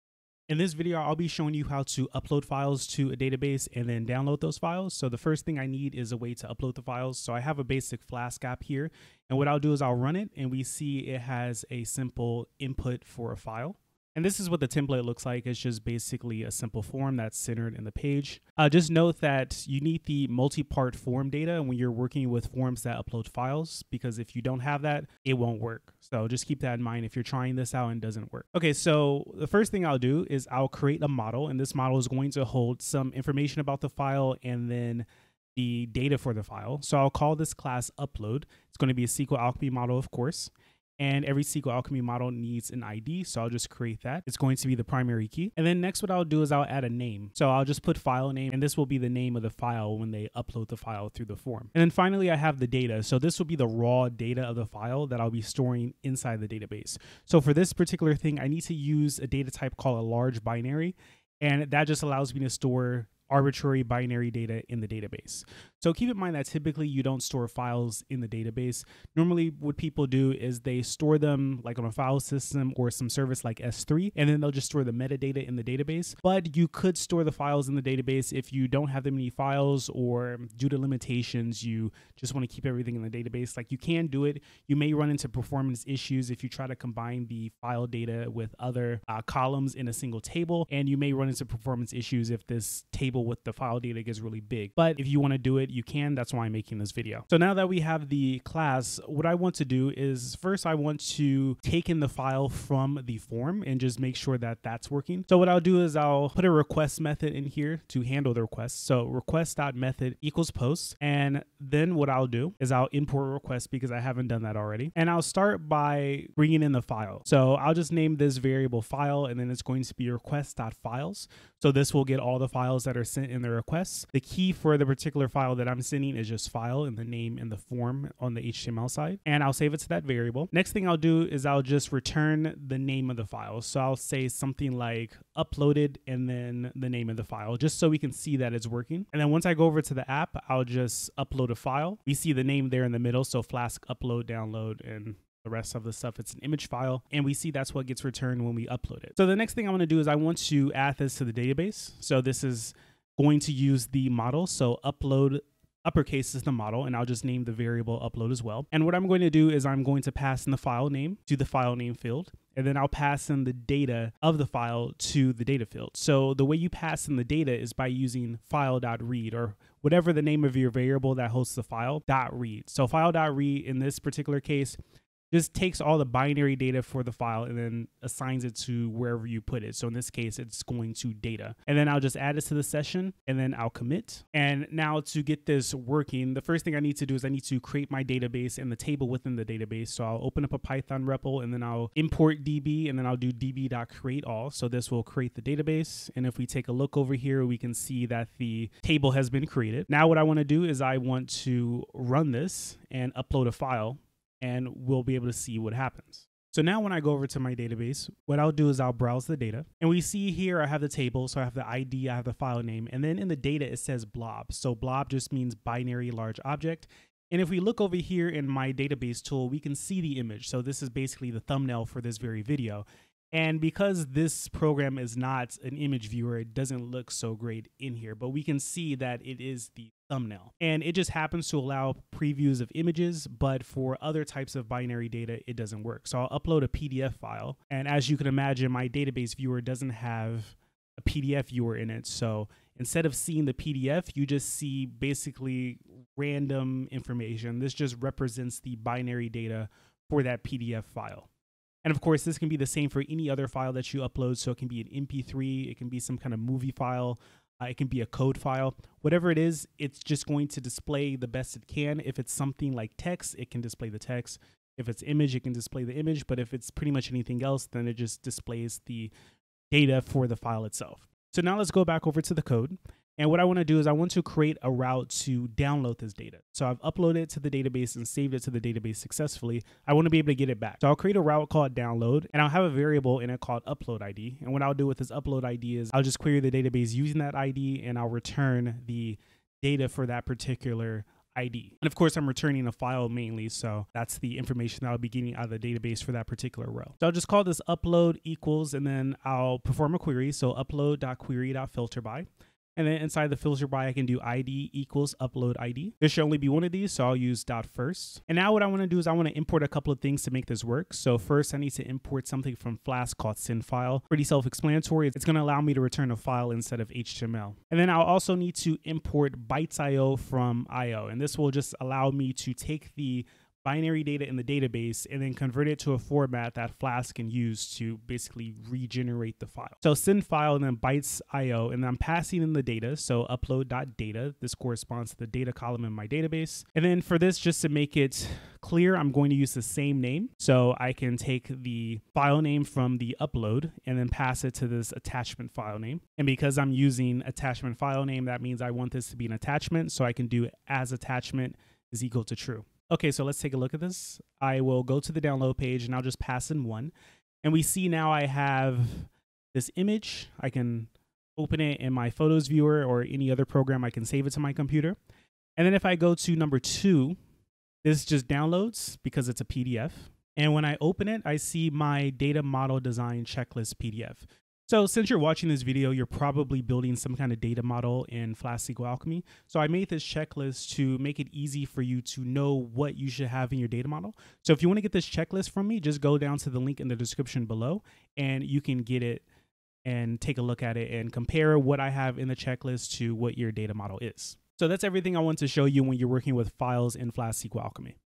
Uploading and Returning Files With a Database in Flask
Learn how to upload files to Flask and save to and retrieve from a BLOB column using SQLAlchemy.